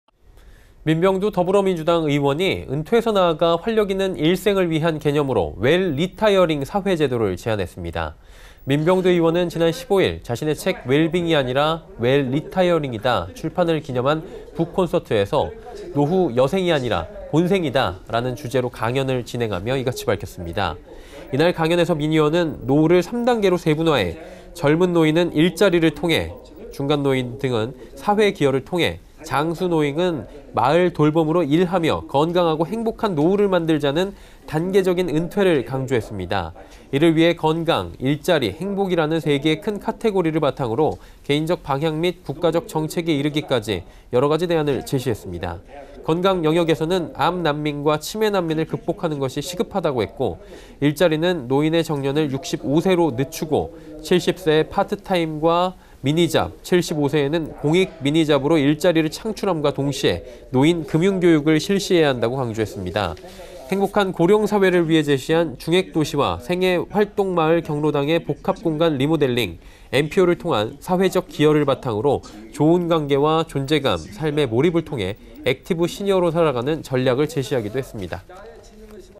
민병두 “초고령사회, 웰빙보다 웰리타이어링”
민병두 더불어민주당 의원이 은퇴에서 나아가 활력있는 일생을 위한 개념으로 웰리타이어링(well-retiring) 사회제도를 제안했습니다.

민병두 의원은 지난 15일 자신의 책 ‘웰빙이 아니라 웰리타이어링이다’ 출판을 기념한 북콘서트에서 ‘노후, 여생이 아니라 본생이다’라는 주제로 강연을 진행하며 이같이 밝혔습니다.

이날 강연에서 민 의원은 노후를 3단계로 세분화해 젊은 노인(65세-75세)은 일자리를 통해, 중간 노인(75-85세)은 NPO 등의 사회 기여를 통해, 장수 노인(85세 이상)은 마을 돌봄으로 일 하며 건강하고 행복한 노후를 만들자는 단계적 은퇴를 강조했습니다.

이를 위해 건강, 일자리, 행복이라는 3개의 큰 카테고리를 바탕으로 개인적 방향 및 국가적 정책에 이르기까지 여러 대안을 제시했습니다.

건강 영역에서는 암난민과 치매난민을 극복하는 것이 시급하다고 했고, 일자리는 노인의 정년을 65세로 늦추고 70세에 파트타임과 미디잡, 75세에 공익 미니잡으로 일자리를 창출함과 동시에 노인 금융교육을 실시해야 한다고 강조했습니다.

행복한 고령사회를 위해 제시한 중핵도시와 생애활동마을(CCRC), 경로당의 복합공간 리모델링, NPO를 통한 사회적 기여를 바탕으로 좋은 관계, 존재감, 삶의 몰입을 통해 액티브 시니어로 살아가는 전략을 제시하기도 했습니다./ 정훈규기자